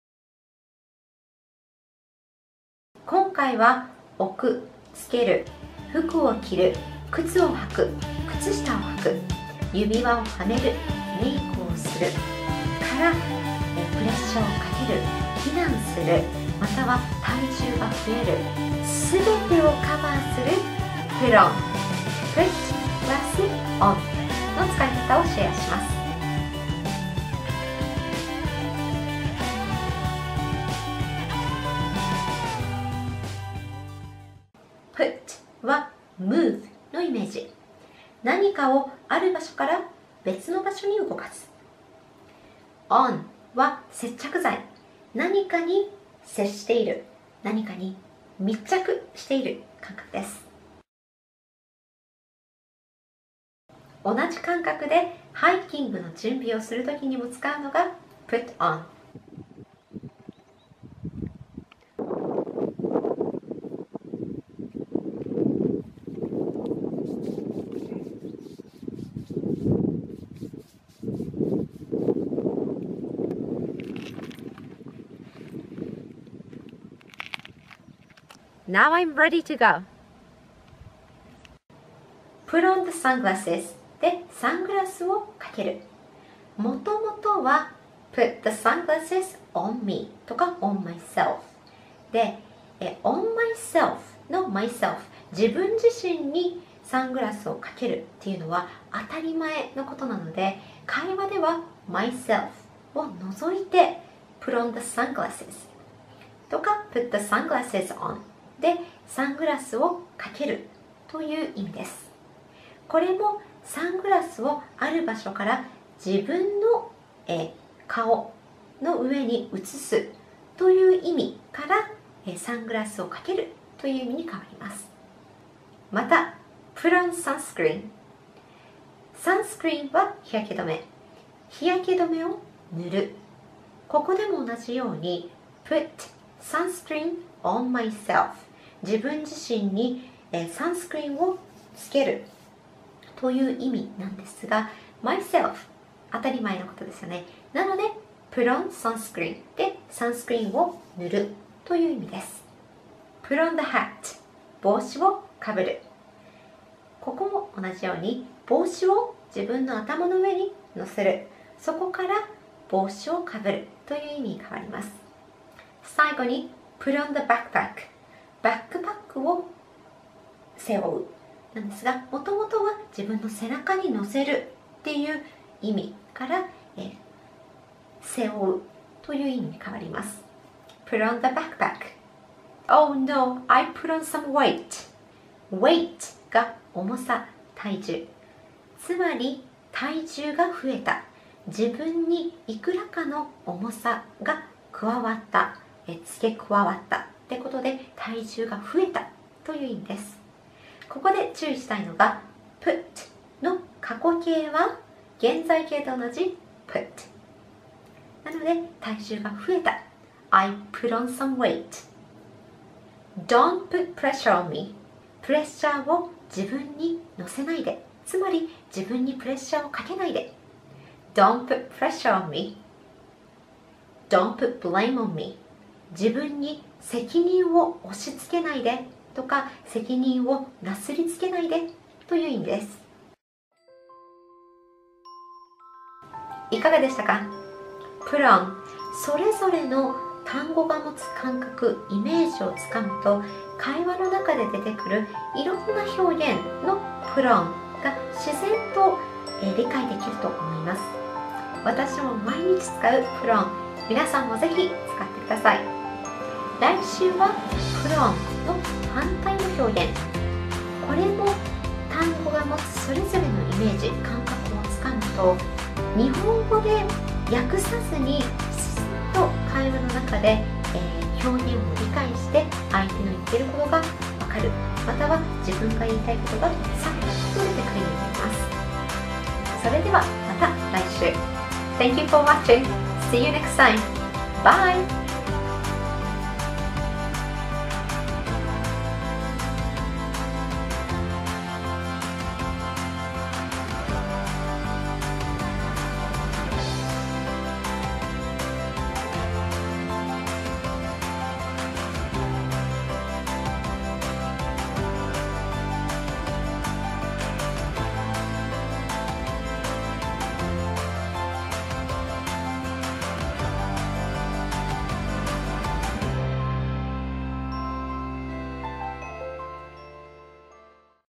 PUT | 英単語＆英語表現の覚え方 [#77]
Put ＿ on ＿
「__に＿を置く」 → 「＿に＿を（身に）着ける」

【この動画に出てくる例文】
★ Put a spider on my shoulder
★ Put on the sunglasses / put the sunglasses on
★ Put on sunscreen / put sunscreen on
★ Put on the hat / put on the hat
★ Put on the backpack / put on the backpack
★ I put on some weight.
★ Don't put pressure on me.
★ Don't put blame on me.

"PUT"の他の表現
★ Put your hands up/down!
★ Put away your toys!
★ I’ve been putting it off. （it: やるべきこと）
★ I can't put my thoughts into words.
★ I can't put it down.（it: 本など）
★ When’s the meeting? Thursday @ 10? Okay, I’ll put it in my calendar.
★ Put (some) music on.
などなど、単語の感覚をつかむと英語を英語のまま理解できる！